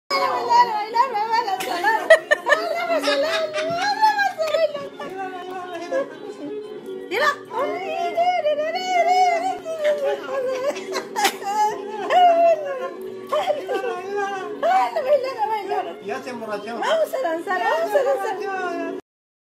Baby girl hype dance
A cute baby girl dancing very excited in her grandma arms.